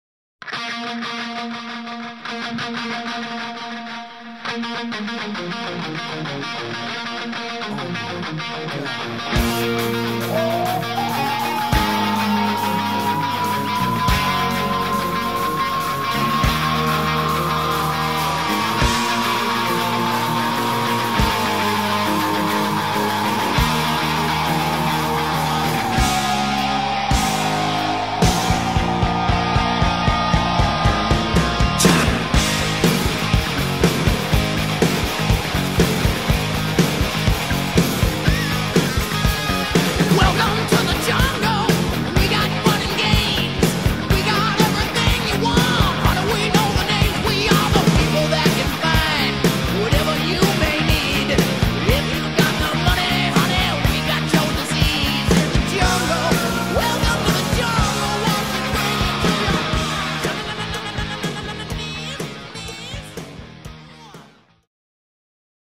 Water Tower - Greater Sudbury - DJI Phantom 2 Vision Plus
Nothing says Sudbury like a good ole’ rusty water tower. Shot with my DJI Phantom 2 Vision Plus near Pearl Street, Greater Sudbury, Ontario.